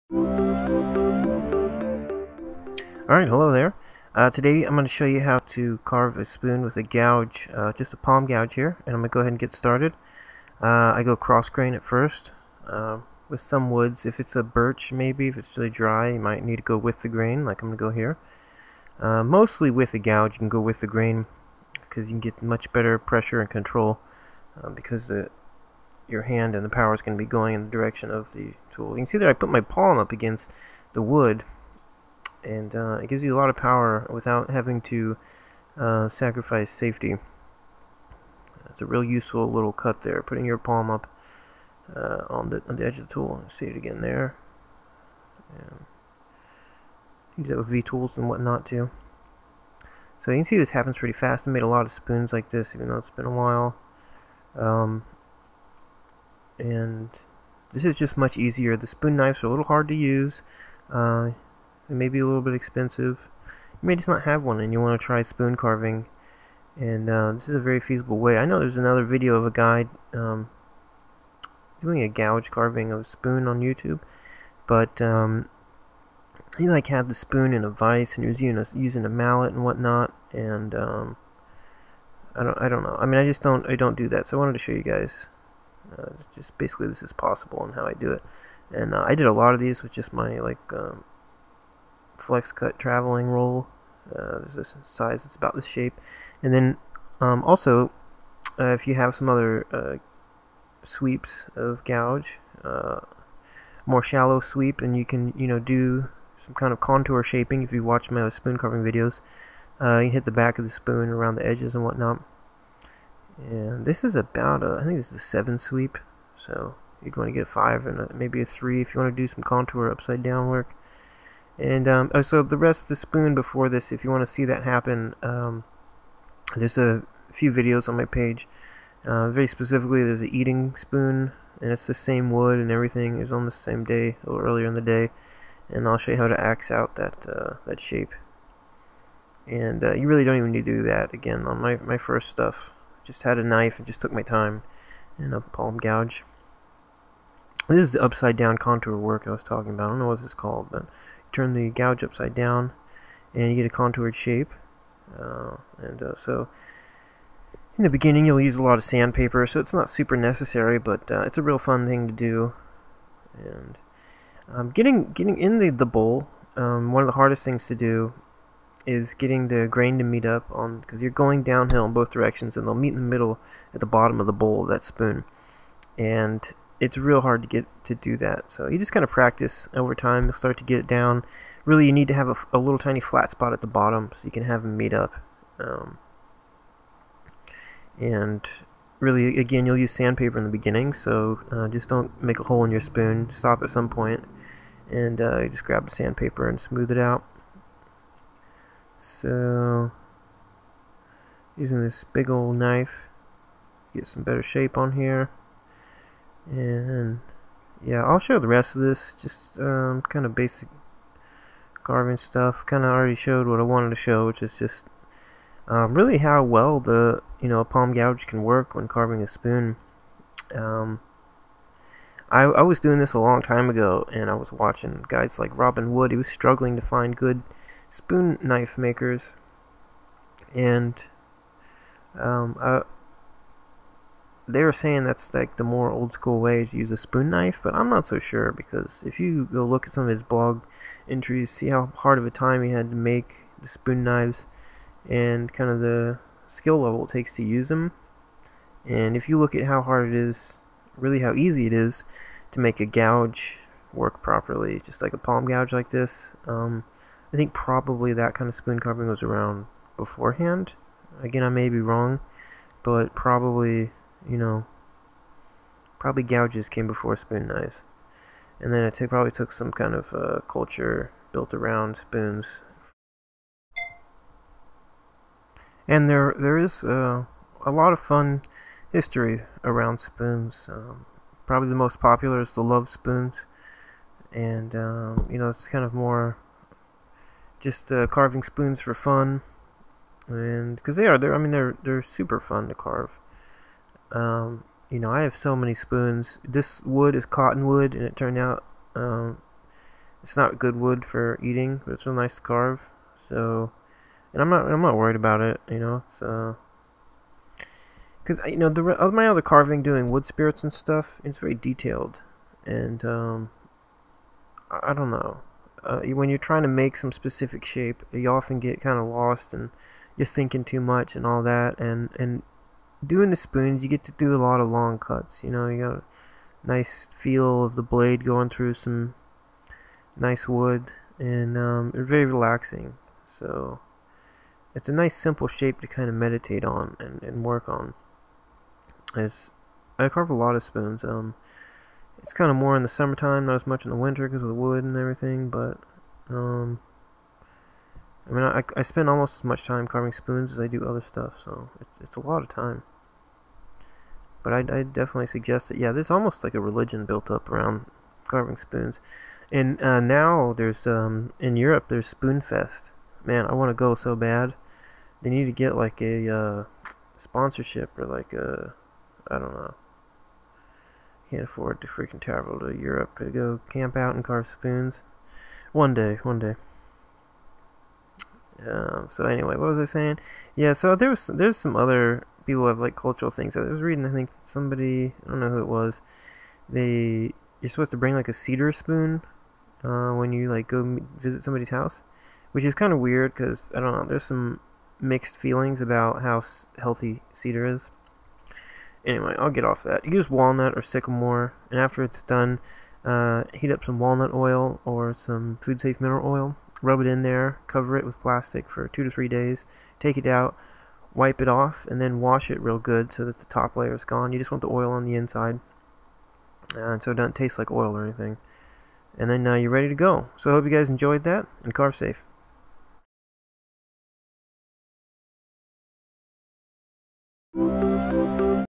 How To Carve A Spoon With a Gouge (palm gouge and knife)
Here I show you how i carve a spoon's bowl with a palm gouge. to see how i axe out the shape up to the start check out my 'eating spoon' video.